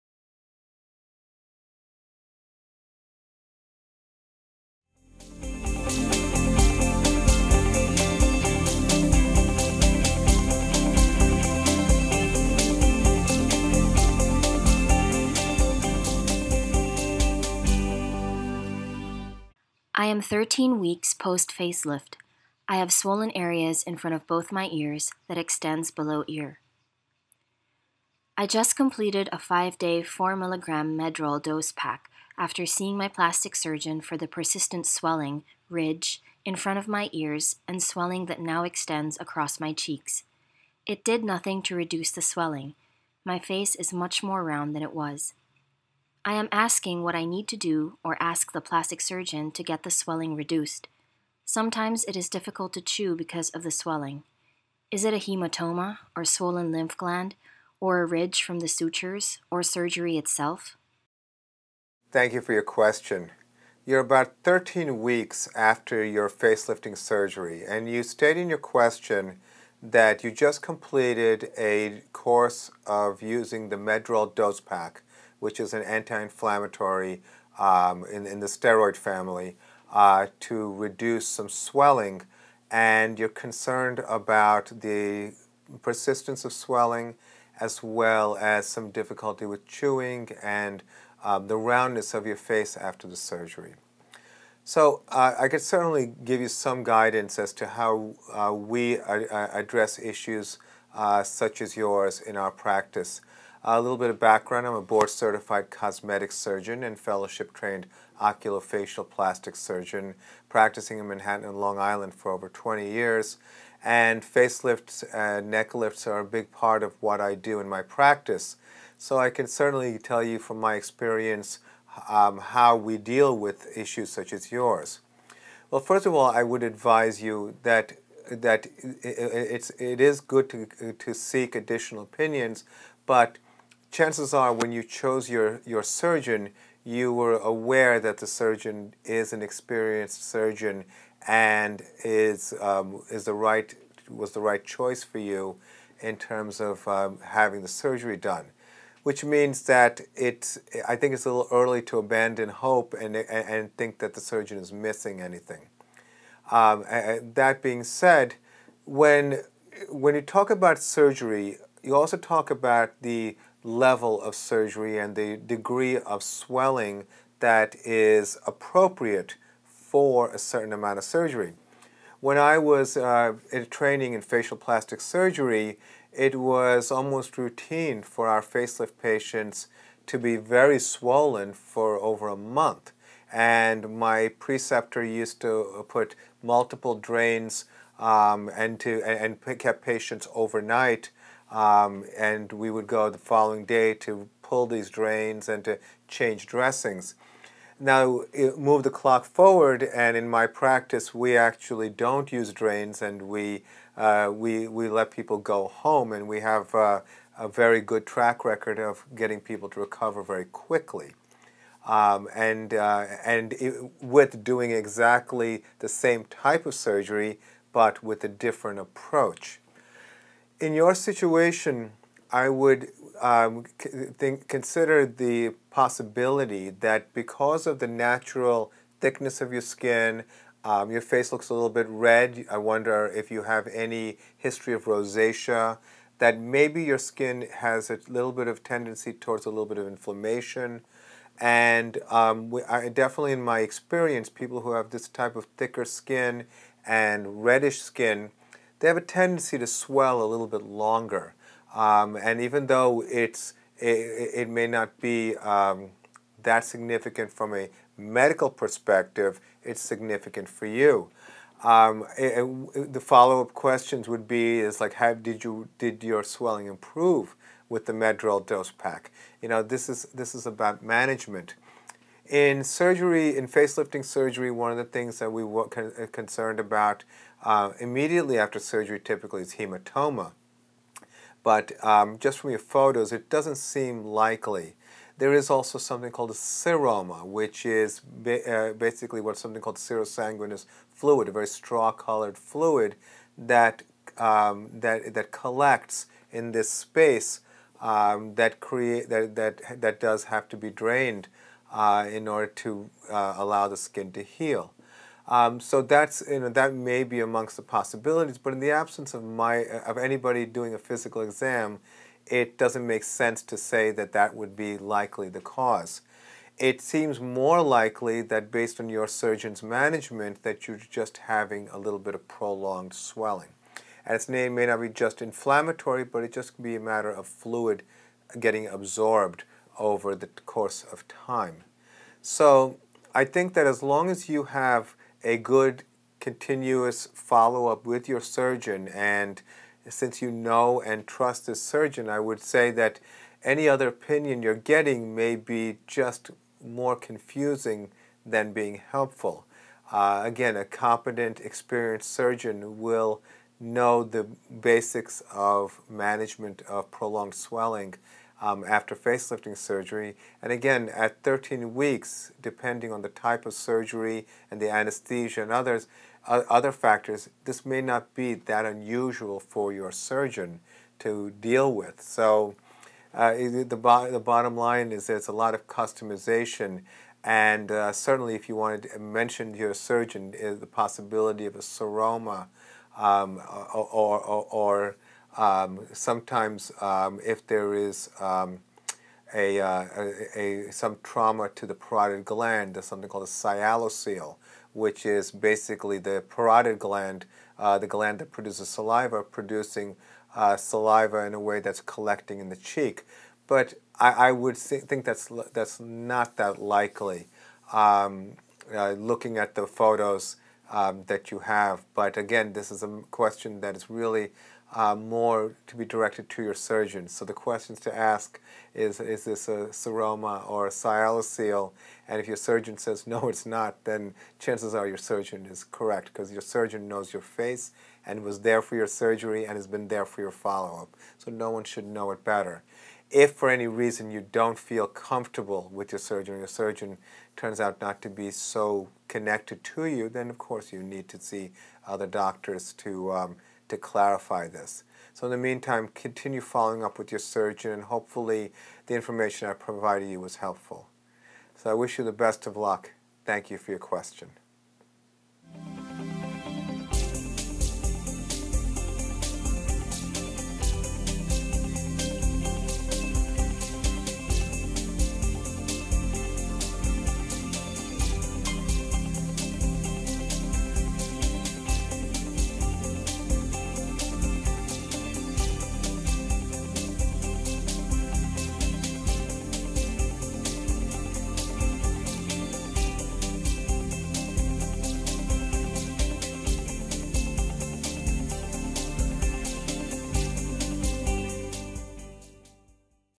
Contributing Factors of Extended Swelling after a Facelift
13 weeks after a facelift, a lady is still swollen in front of her ears and across cheeks, which is making it difficult for her to chew. She just finished a 5-day cycle of the inflammatory Medrol, but it did little to reduce her swelling. She asks if it is a swollen lymph gland, hematoma, or the ridge from the sutures causing the swelling.

In this video, New York Oculofacial plastic surgeon Amiya Prasad, M.D. reviews this lady's pictures, and lists the possible causes of swelling, and how he avoids an extended healing phase in his practice:

1:55 - Samples of facelifts and neck lifts performed by Dr. Prasad
2:10 - The unique insight of the original surgeon, and why their guidance is valuable
2:44 - Why extent of swelling is directly associated with extent of surgery
2:58 - How swelling was extended during Dr. Prasad’s training years, with patients staying overnight, needing multiple drains after surgery, and swelling lasting over a month
3:20 - How Dr. Prasad now performs the same type of facelift surgery, but with less swelling and faster recovery time
3:44 - Natural skin thickness, reddish skin, or a history of rosacea all contribute to longer swelling
4:35 - How hematoma and seroma contributes to extended swelling, even if these may not be the causes
5:16 - Prolonged swelling due to absorption of fluid during healing
6:00 - Why swelling after 13 weeks may not be unusual, depending on the surgeon
6:30 - Possibility of trauma of the parotid gland producing saliva that collects and causes swelling
6:57 - Why asking specific questions to the surgeon who knows your anatomy can help determine the cause of extended swelling